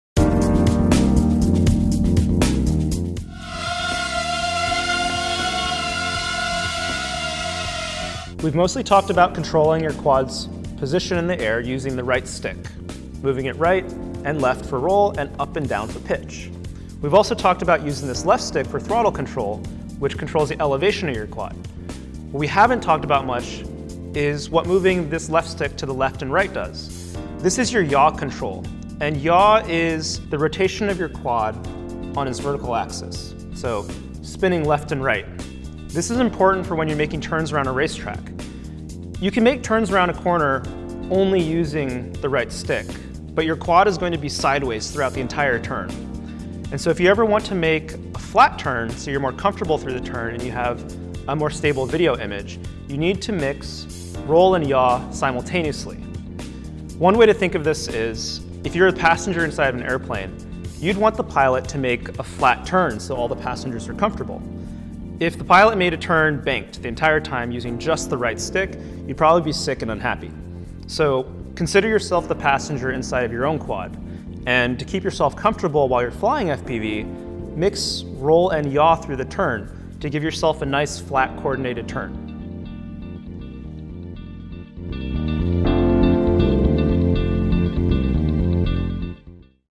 Fat Shark 101 Training, Episode 8: Free Flight in FPV
IN THIS EPISODE: How to dynamically control your quad in FPV mode.